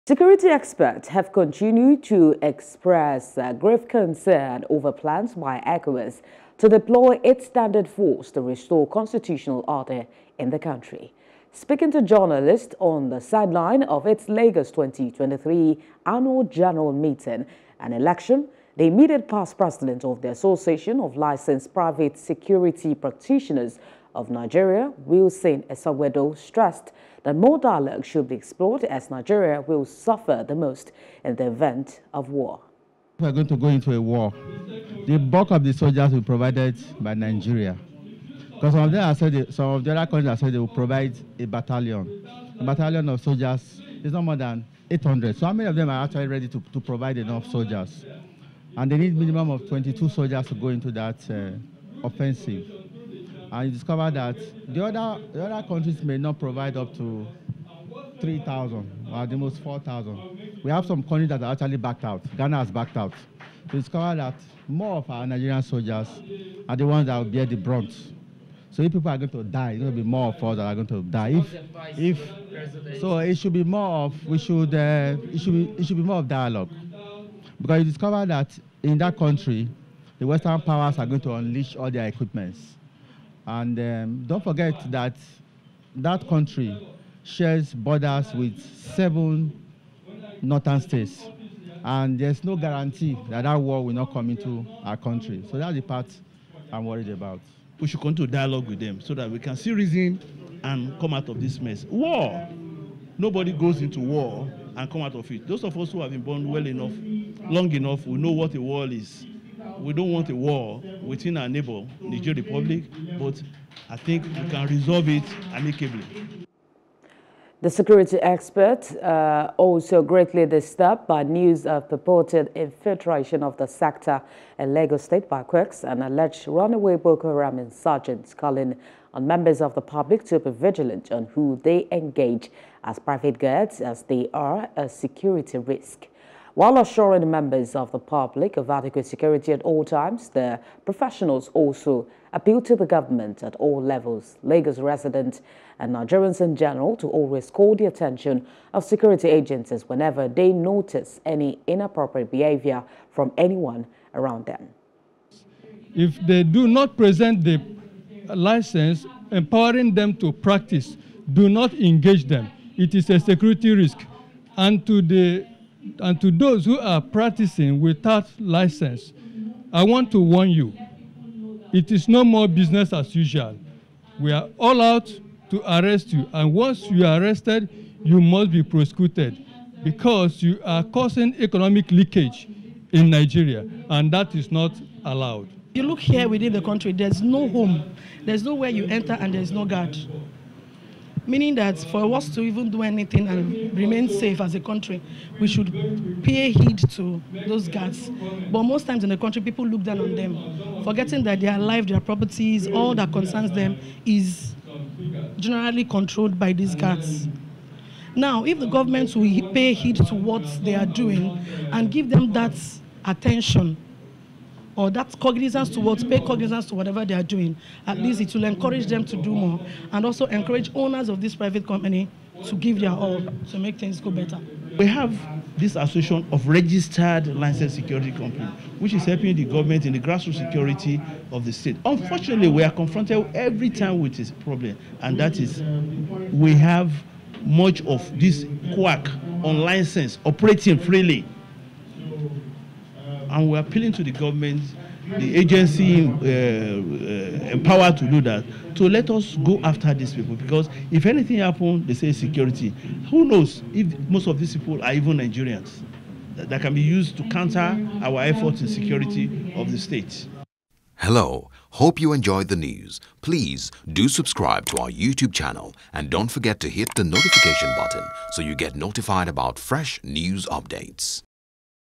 Security Experts Call For Dialogue, Say Nigeria Will Be Worse Hit, In Event Of War
Security experts have continued to express grave concern over plans by ECOWAS to deploy its standby force to restore constitutional order in the country.